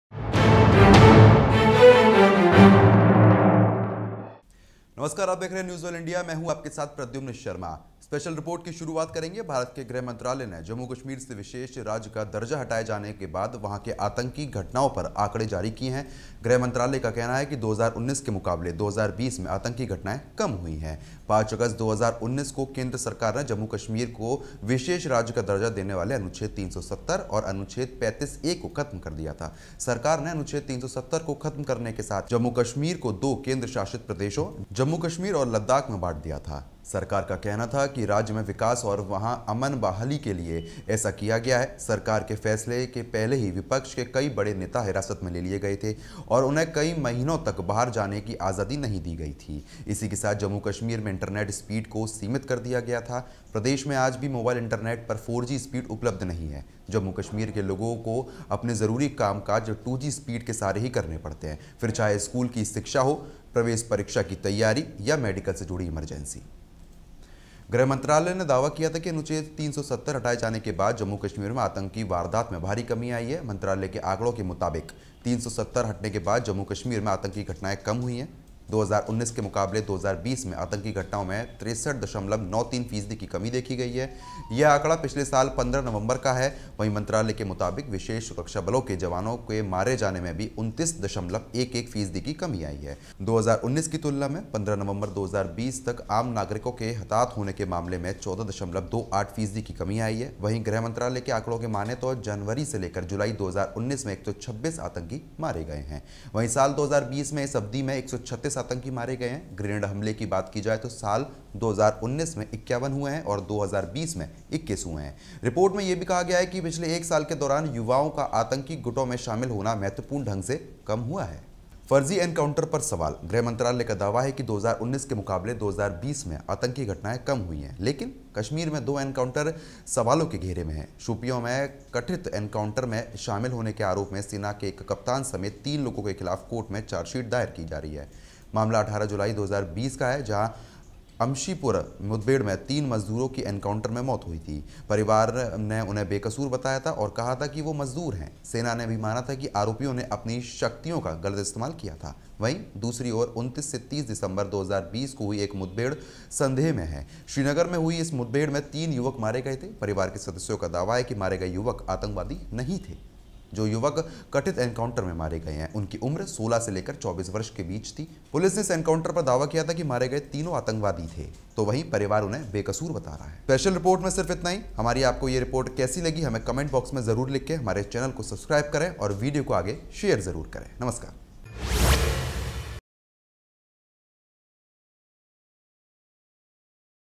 Jammu and Kashmir Witnessed 63.93% Decrease In Suspicious Incidents in 2020 | MHA Report |NWI NEWS
The Kashmir policy of the Government of India has been so successful. This can be gauged from the fact that ever since Article 370 has been removed from Jammu and Kashmir, there has been a huge decline in terrorist incidents in the Valley, this is not what we are saying but the Home Ministry figures. According to information provided by the Ministry of Home Affairs, there has been a decrease of 63.93% in terrorist incidents in Jammu and Kashmir till November 15 in 2020 as compared to 2019.